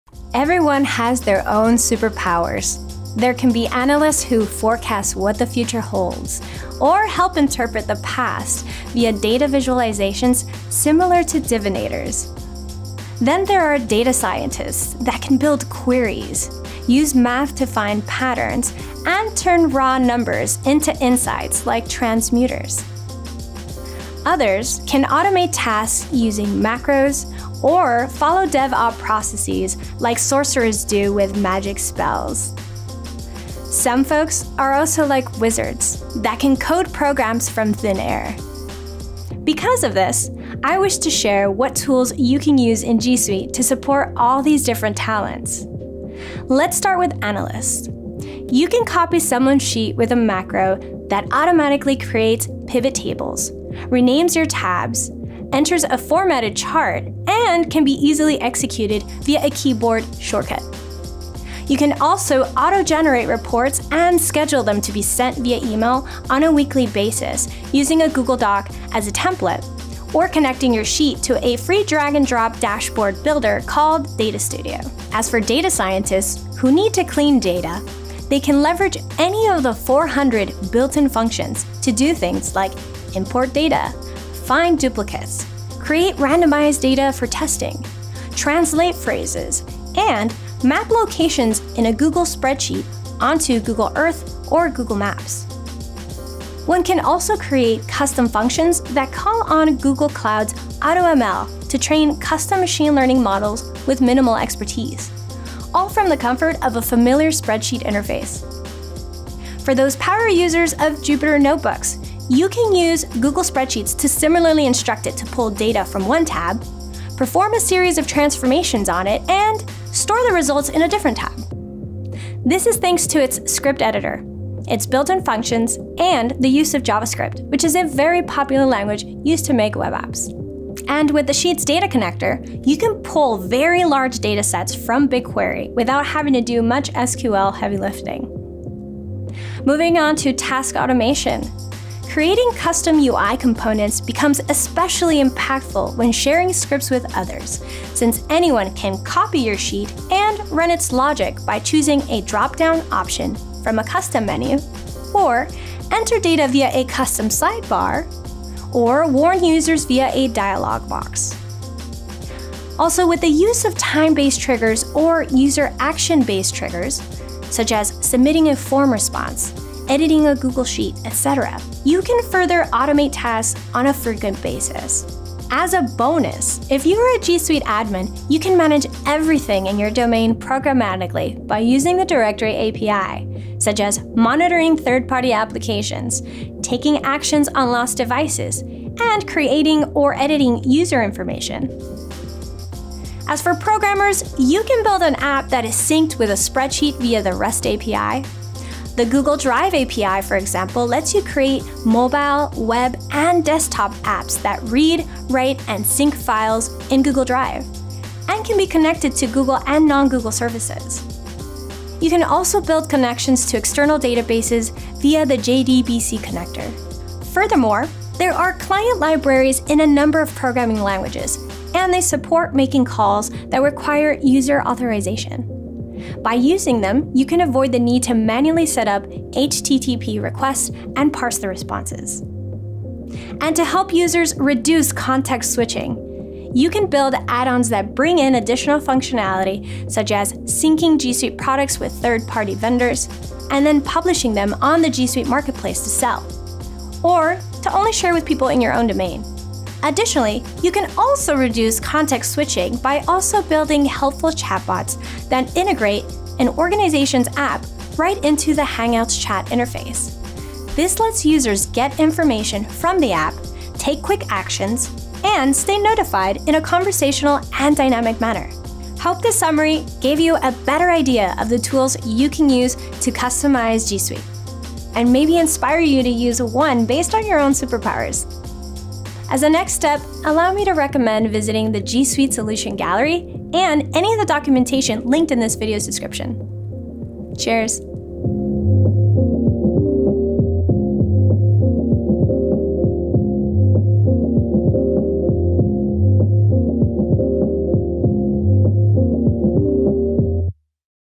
Build it for G Suite
Everyone has their own superpowers. Whether you’re an Data Analyst, Data Scientist, Task Automater, or Programmer, you can use G Suite to support these talents. In this video, we’ll go over what tools you can use to support these different talents.

Product: G Suite, G Suite development, Cloud, full name: AGV;